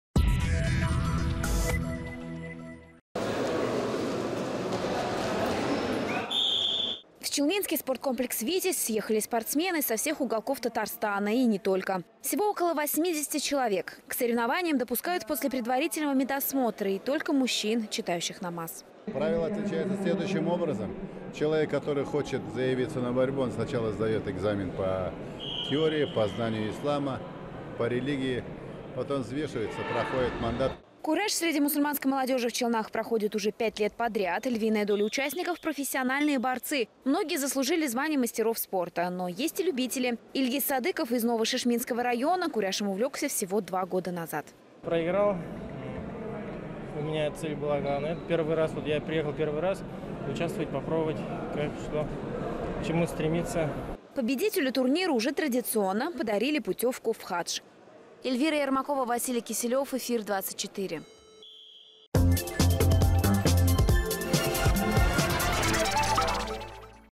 В Набережных Челнах прошел всероссийский турнир по национальной борьбе куреш
Главный приз предоставило Духовное управление мусульман РТ. Победителю турнира по традиции подарили путевку в хадж. Среди участников исключительно молодые люди, исповедующие ислам.